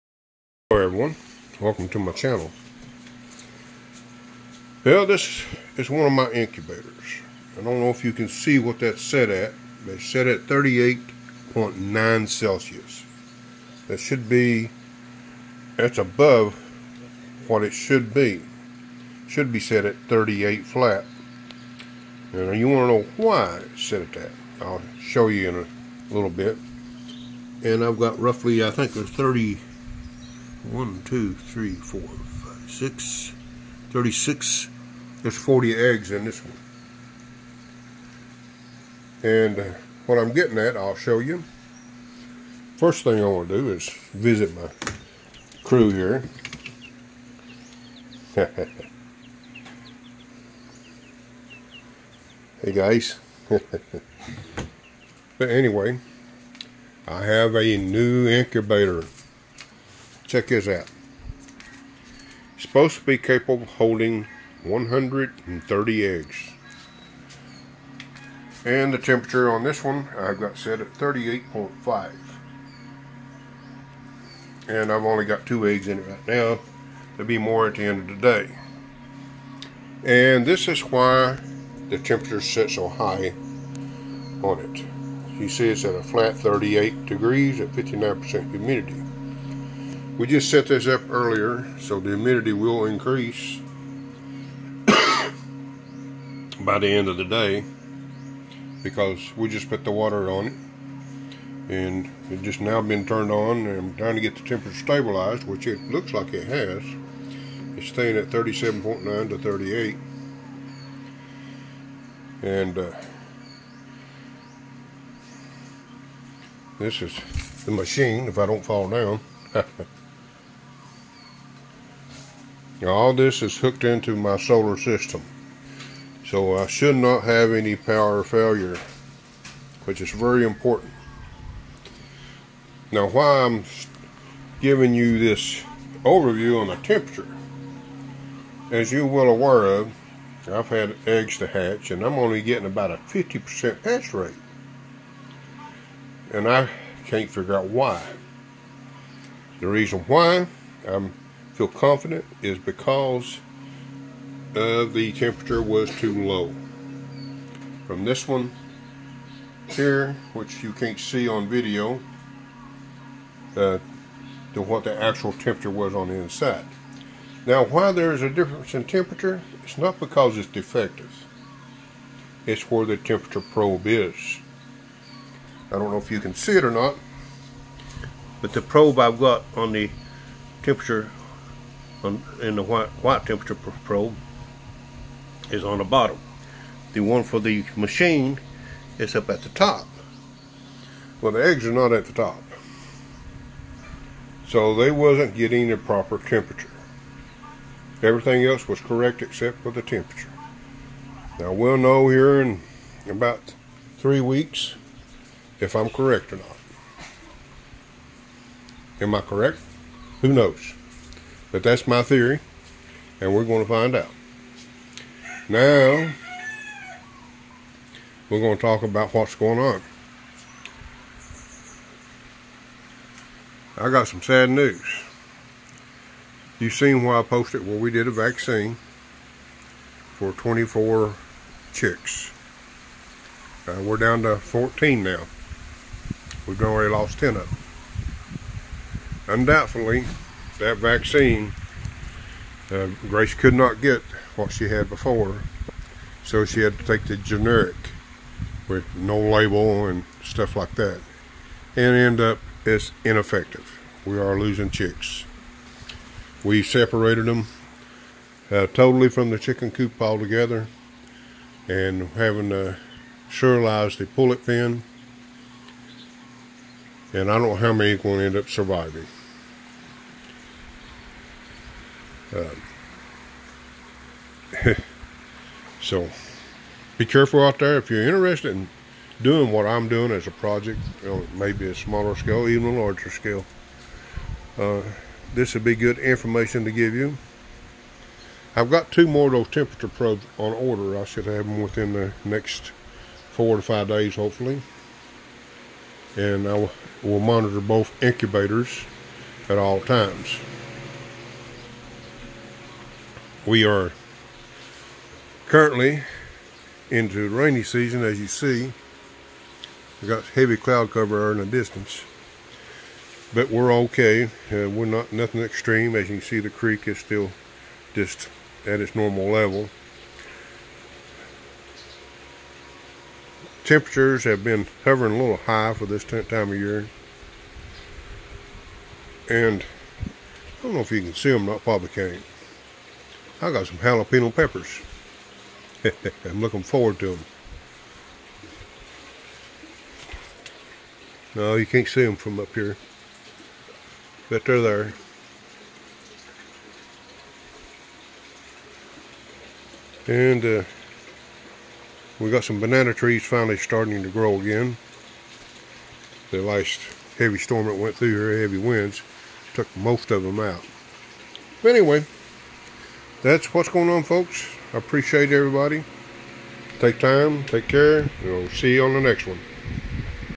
Incubator/ chick's ❤️
Overview of the incubators , what I believe cause the low hatch rate, dead vaccines, dieing chick's again 😳